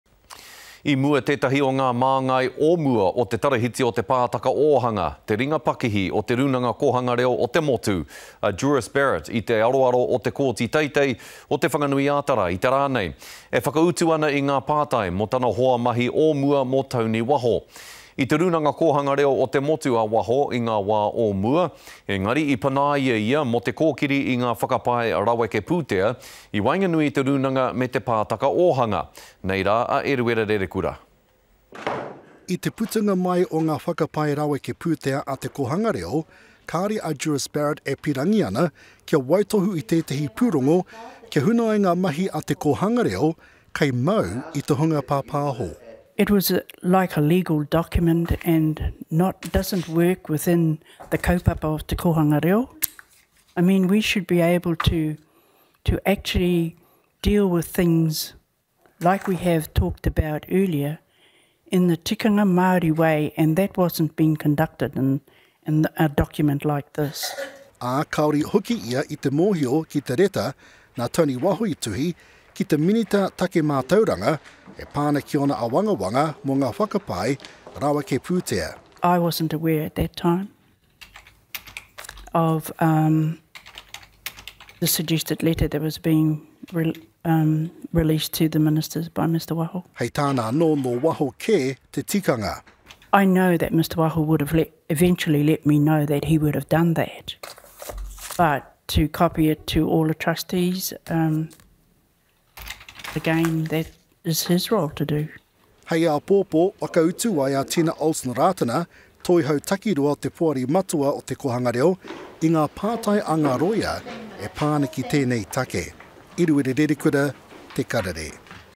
Former Te Pātaka Ōhanga member denies knowing about whistle blowing
A former board member of Te Pātaka Ōhanga Trust, the subsidiary company of the Kōhanga Reo National Trust, Druis Barrett, answered questions in the high court in Wellington today about her dismissal from the board in 2014.The legal battle over allegations of financial impropriety continues in the high court this week. Eruera Rerekura reports.